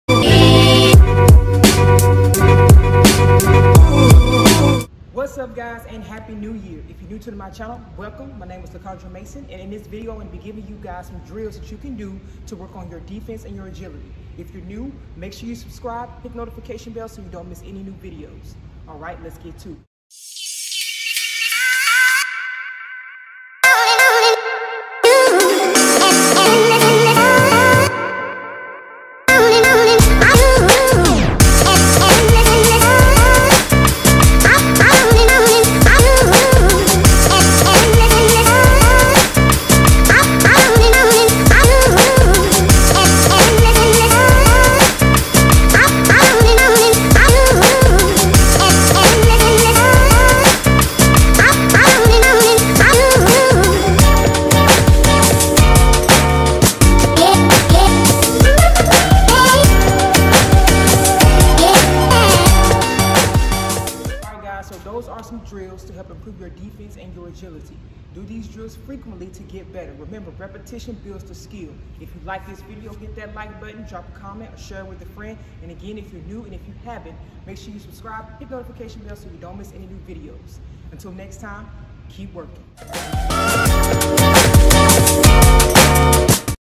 Agility + Defense Drills|CMase Basketball
Do these drills to improve your agility and defense.